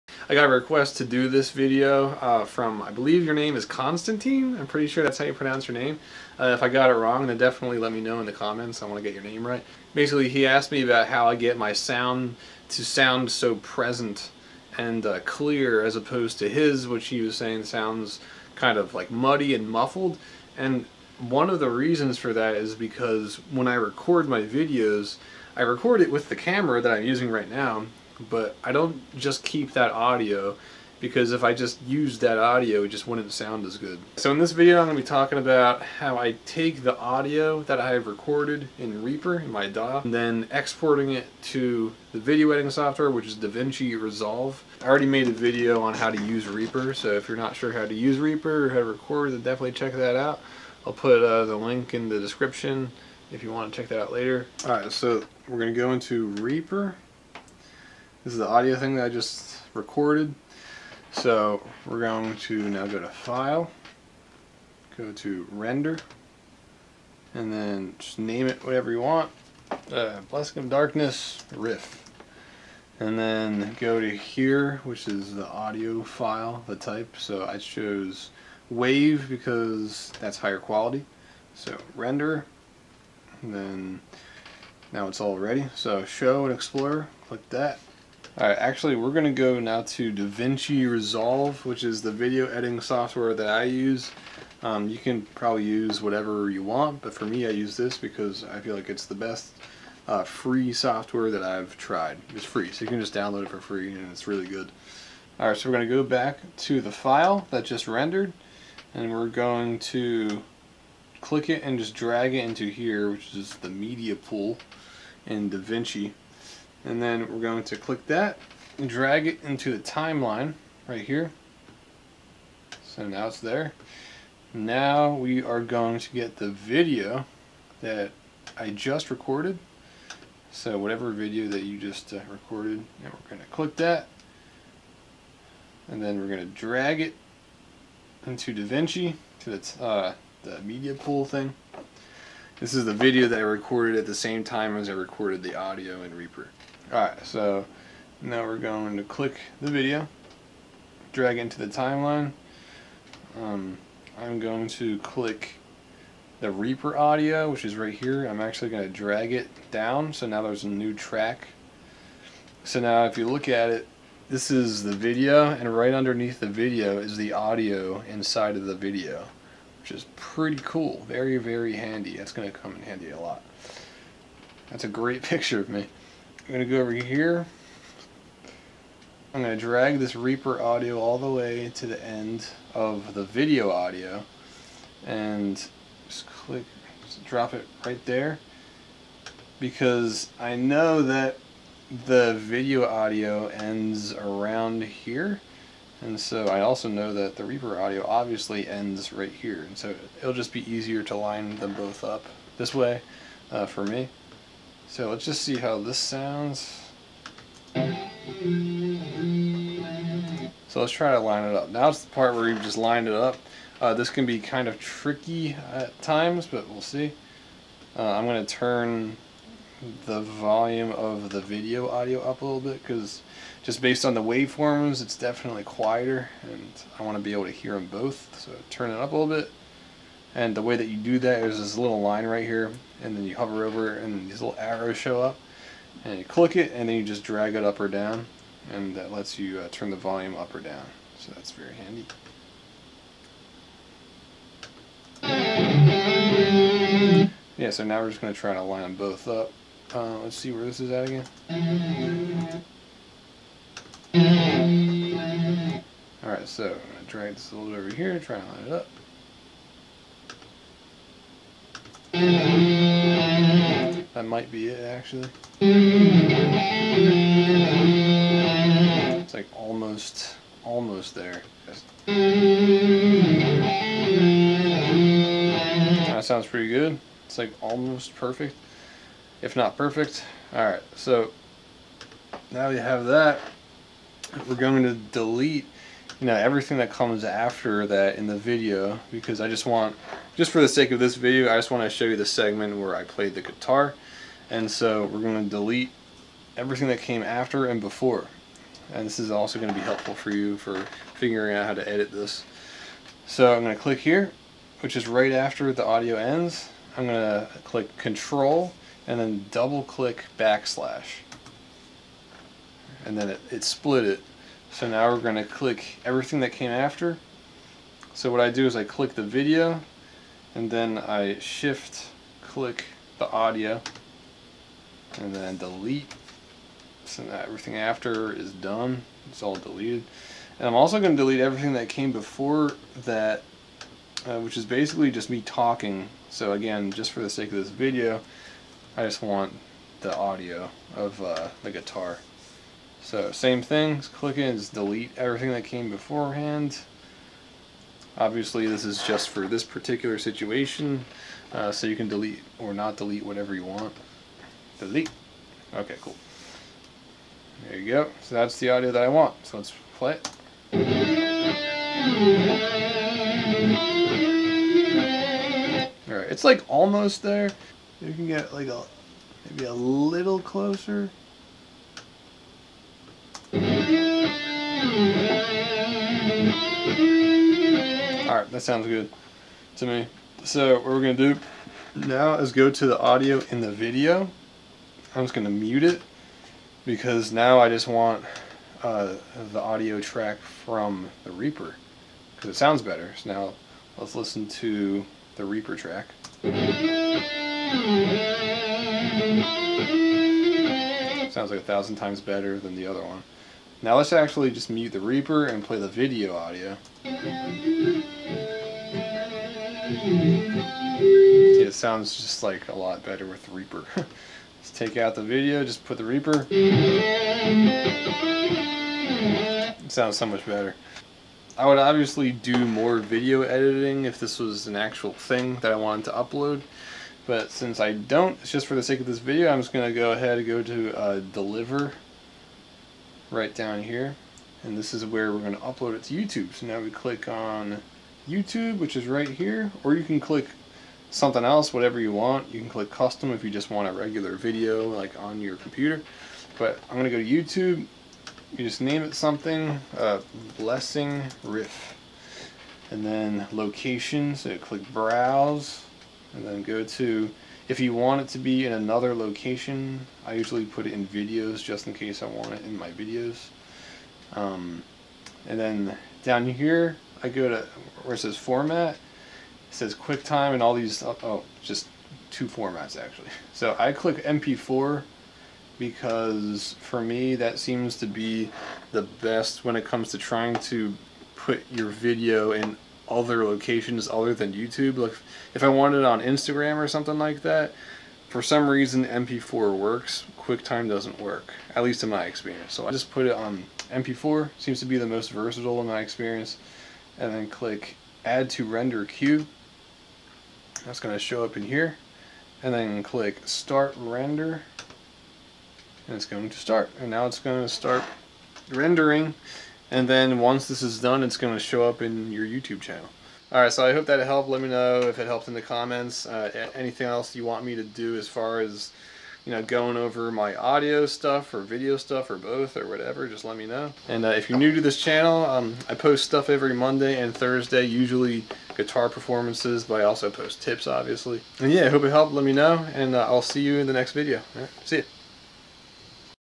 How I Replace Camera Audio with DAW Audio using Reaper and Davinci Resolve 16
In this video I teach you how I replace the audio from my camera with the audio from my DAW (Reaper) using Davinci Resolve 16.  The object is to get a clearer sound in your videos by first recording yourself in your DAW, exporting the audio into the video editing software, and then lining up the audio.  The trickiest pat is lining up the audio from your DAW with the camera audio.  In this video I walk you through my process when it comes to doing all of this, which is the way I do it for all my performance videos.

Really great tip was brought to my attention!  If you want this process to go even faster you can use Autosync which lines the two audio files up automatically.  Just watched this really good video on how to do it and it's super easy.